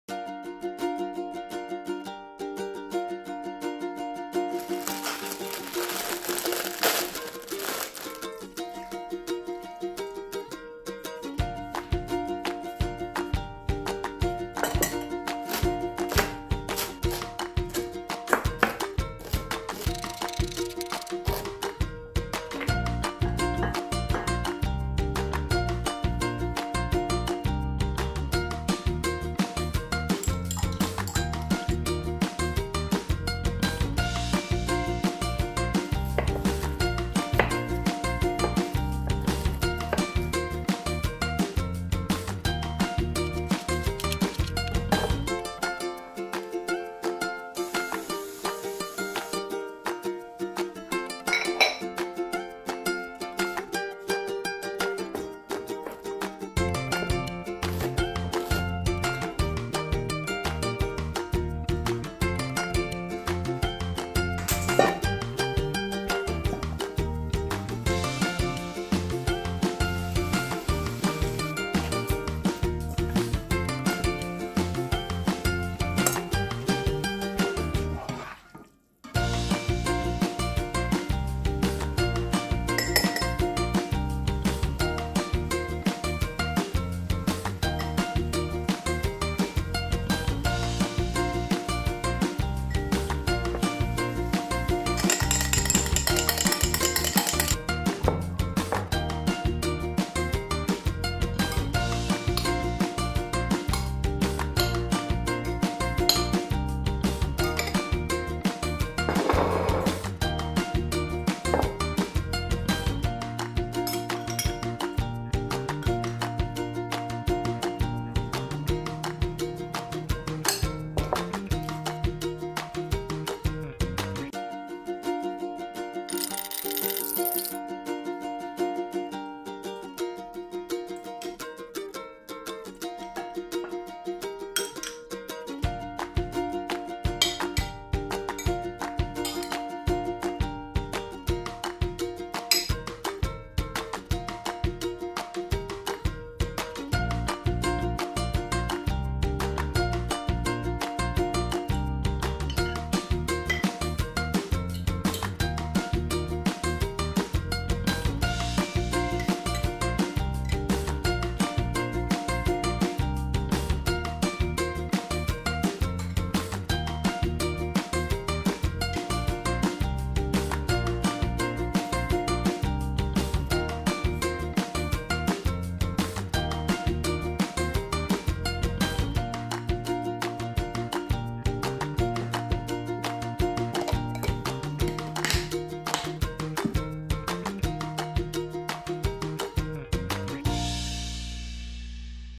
Ground Beef & Veggie Ramen | What's For Lunch?
For today's lunch, we've got a ground beef & veggie ramen (without the broth so it travels better) and steamed broccoli with a homemade sauce.

ABOUT
I love cooking for my family, and now, for you! Please join me each week for some tasty lunch ideas. :)

VIDEO CREDITS
Music used with permission via Envato Elements